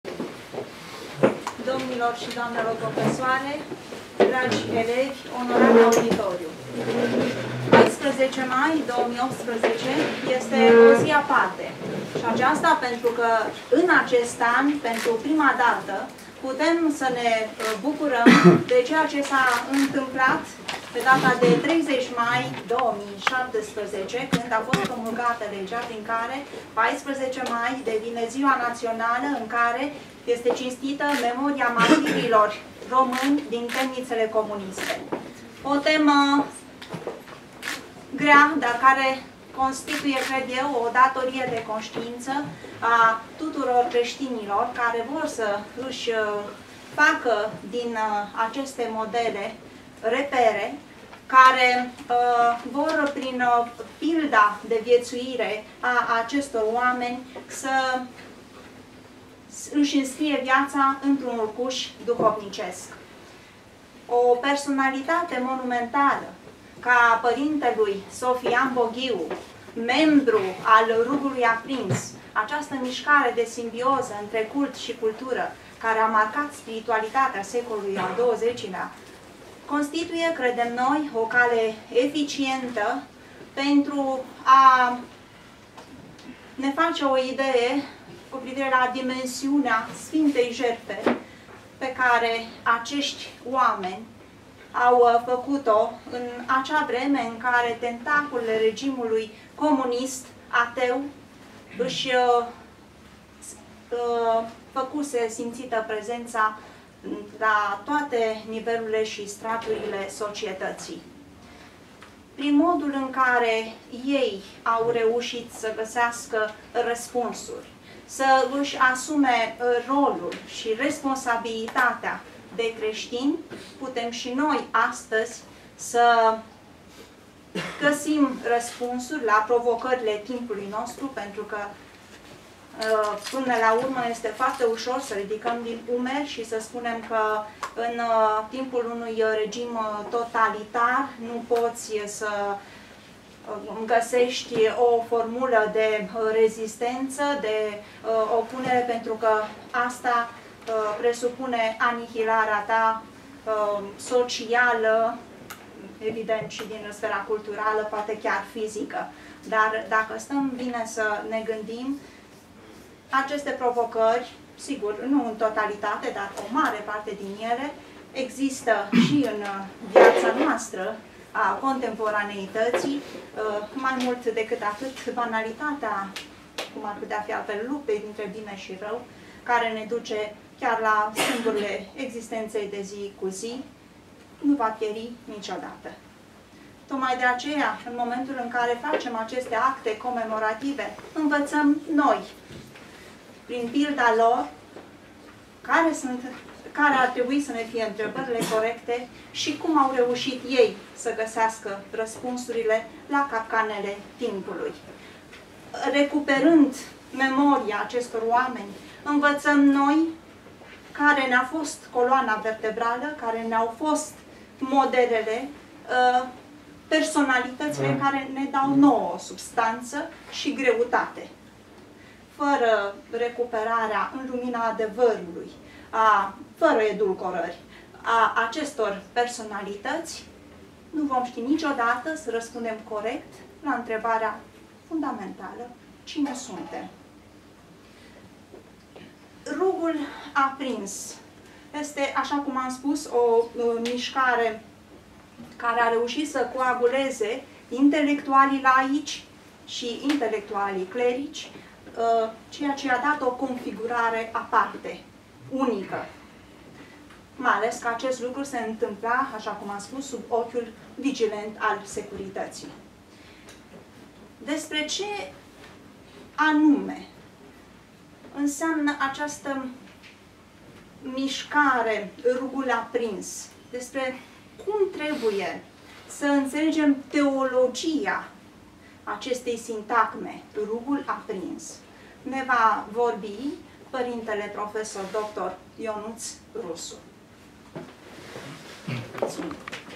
(03) Lect.univ.dr. Carmen Ciornea - Introd. - Arhim. Sofian Boghiu și taina Rugului Aprins
Luni, 14 mai, începând cu ora 12.00, la Curtea Brâncovenească, a avut loc conferința "Arhimandritul Sofian Boghiu și taina Rugului Aprins". Evenimentul a avut loc cu prilejul zilei naționale de cinstire a martirilor români din temnițele comuniste, sărbătorită pe 14 mai.

Au susținut prelegeri: Înaltpreasfințitul Teodosie, Arhiepiscopul Tomisului, Pr.prof.dr. Ionuț Rusu, Pr.prof.dr. Petre Oprea și Lect.univ.dr. Carmen Ciornea, cadru didactic asociat al Facultății de Teologie Ortodoxă din cadrul Universității "Ovidius".

Programul a conținut și momente artistice susținute de către membrii Cercului teatral și de către Grupul psaltic "Sfântul Cuvios Dionisie Exiguul", din cadrul Seminarului Teologic "Sfântul Dionisie Exiguul".

Mai jos, fotografii, alături de înregistrarea video și audio a evenimentului.